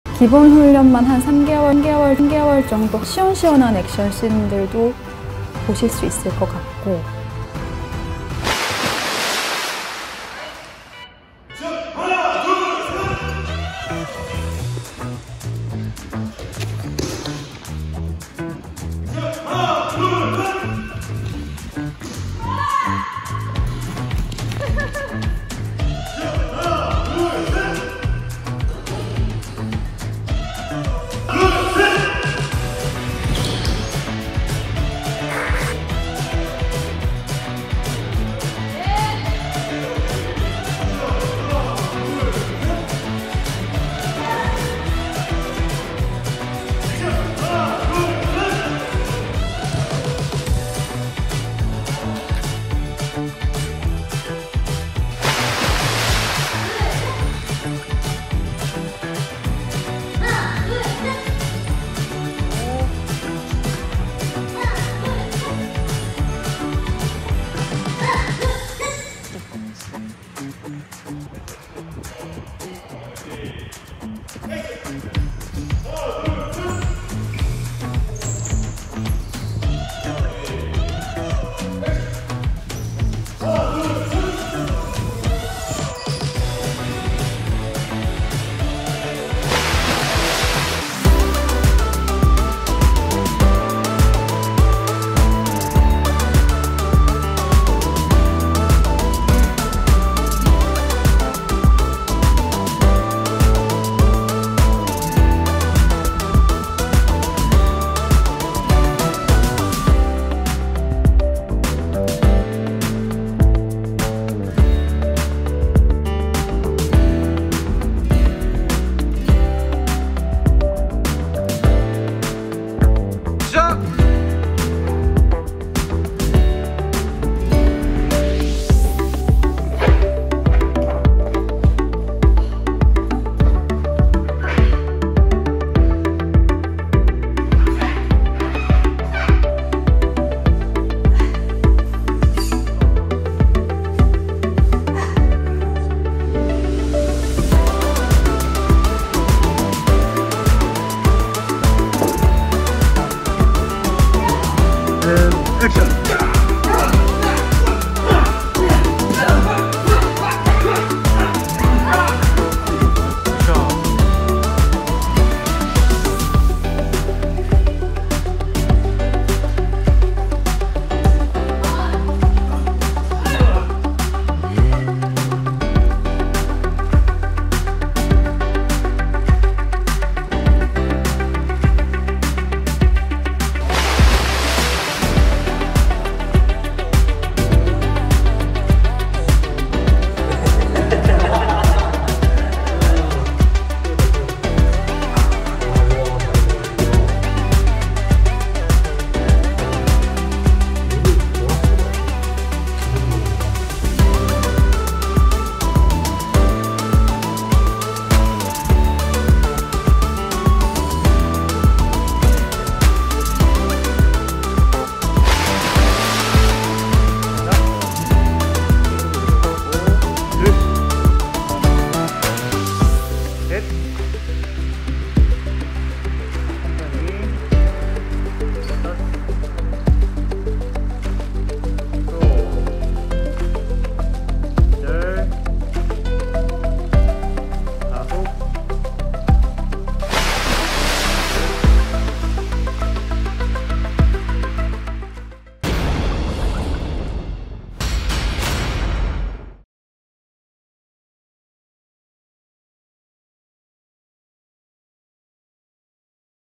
🚨효랑단 필수 시청🚨 해적 효주로 거듭나는 스펙타클 언빌리버블 액션스쿨 (검술/와이어/수중/헬스 트레이닝)
🚨효랑단 필수 시청🚨
해적 효주로 거듭나는 스펙타클 언빌리버블 액션스쿨 (검술/와이어/수중/헬스 트레이닝)

기본 훈련만 3개월, 육해공을 넘나들며 연습한 효주의 액션 영상을 공개합니다!